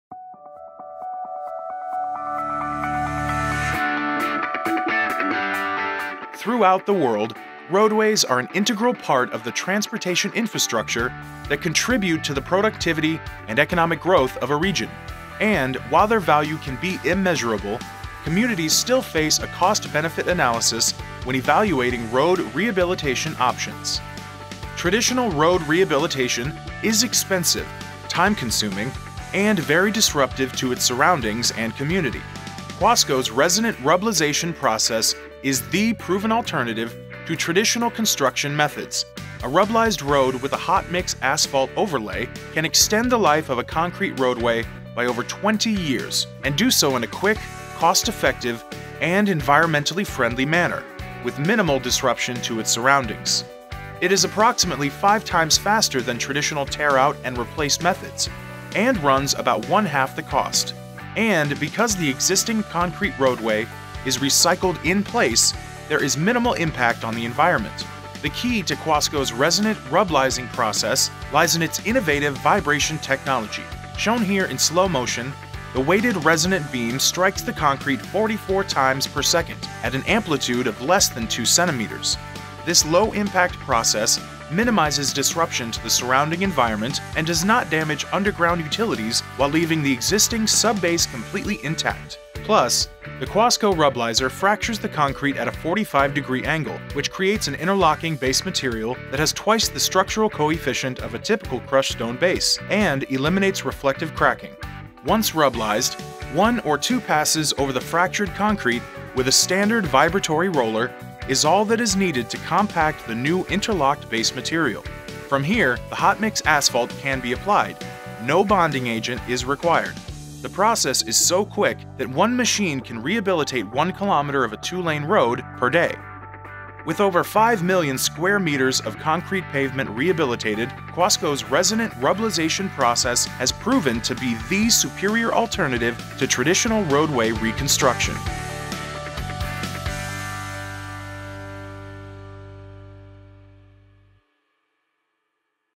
Quasco - Resonant Pavement Breakers
Quasco's innovative Rubblization Process provides an attractive alternative to the traditional techniques of tearing out and replacing the concrete or applying asphalt directly over the existing concrete base.